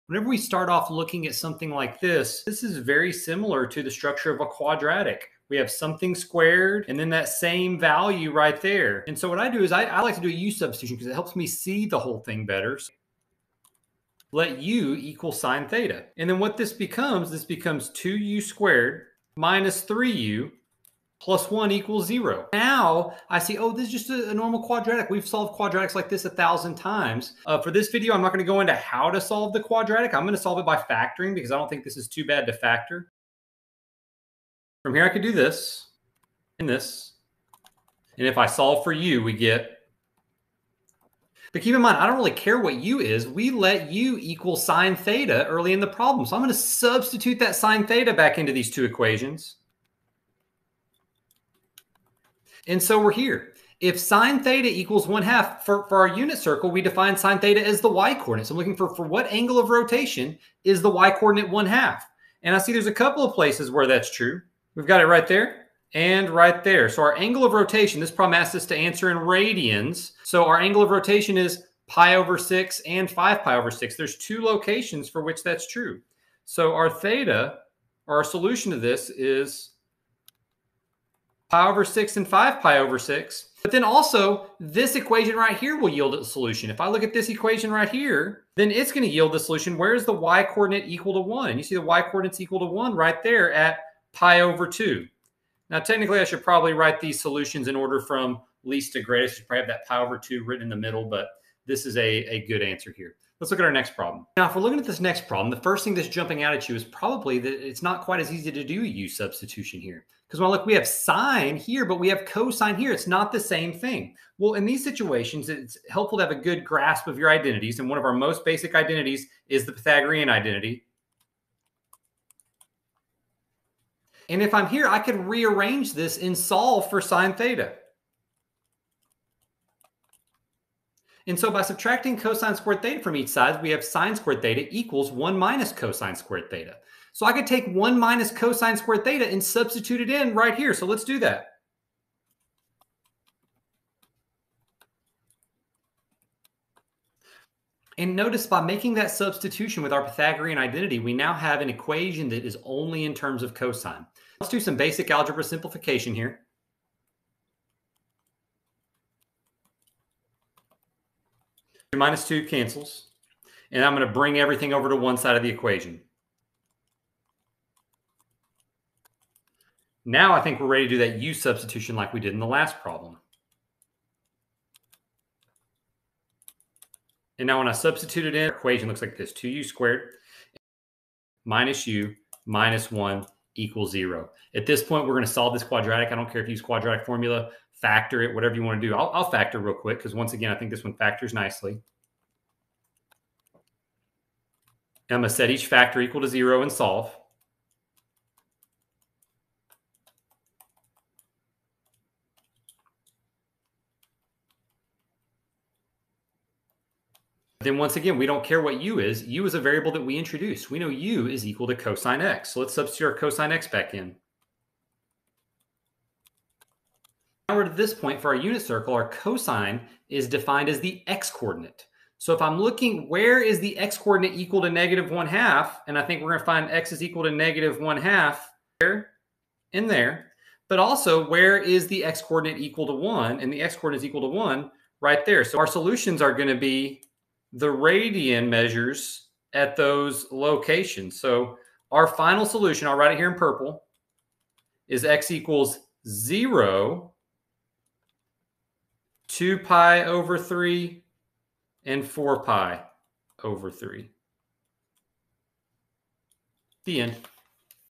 Solve Quadratic Trig Equations
How do you solve trig equations that have something squared in them? In this video I show how to use a U-substitution to rewrite trigonometric equations as quadratics, which makes them easier to solve. We use the unit circle to solve these particular problems.

TIMESTAMPS:
0:00 Example 1
1:43 - Example 2 (requires Pythagorean Identity)